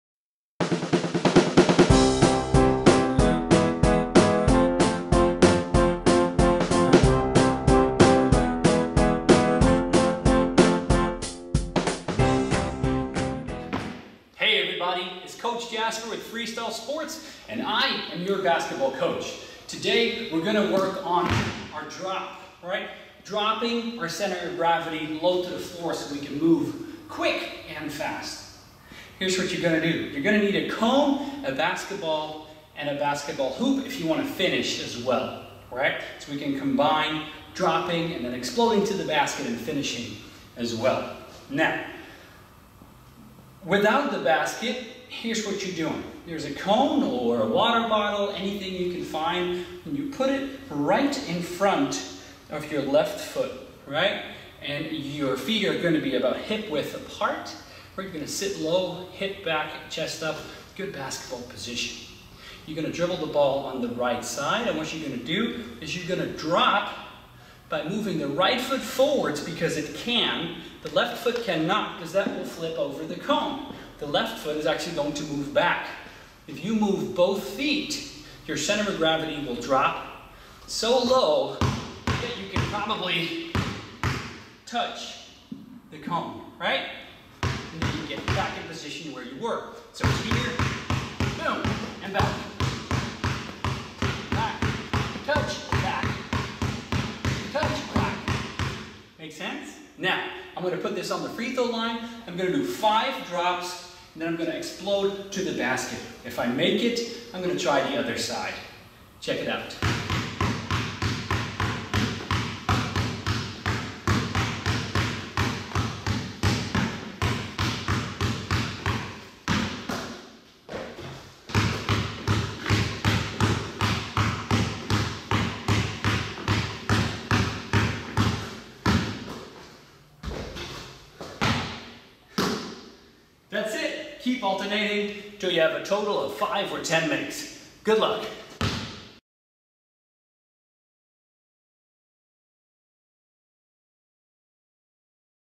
The Drop Drill
Practice dropping your center of gravity helps you move quick and fast. You need a basketball and a cone (and a hoop if you want to practice finishing as well). Move one foot forwards, the other back at the same time and touch the cone.

Work Hard.
Play Smart.
Have Fun.